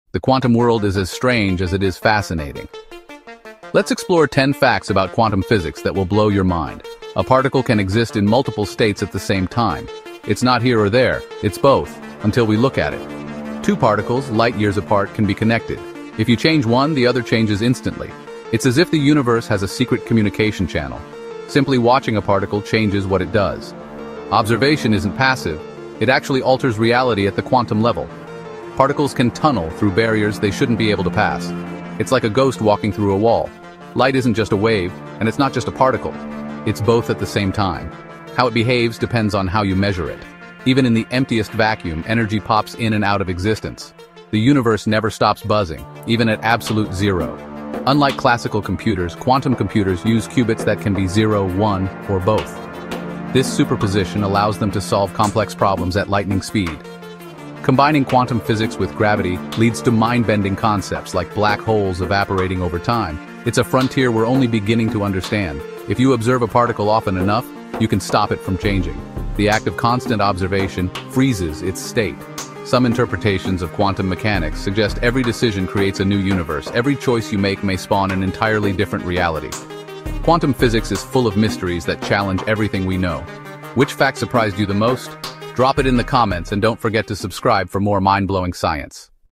Mind-Bending Facts About Quantum Physics 🧠⚛️
Quantum physics is stranger than science fiction — and it’s real.
These 10 facts take you inside the mysterious world of quantum mechanics, from particles that exist in multiple states to quantum entanglement’s spooky action at a distance.

What’s in the video:
Superposition: Particles in multiple places at once
Entanglement: The universe’s secret communication channel
The Observer Effect: How watching a particle changes reality
Quantum Computing: Qubits solving problems at lightning speed
The Multiverse Hypothesis: Every choice spawning new universes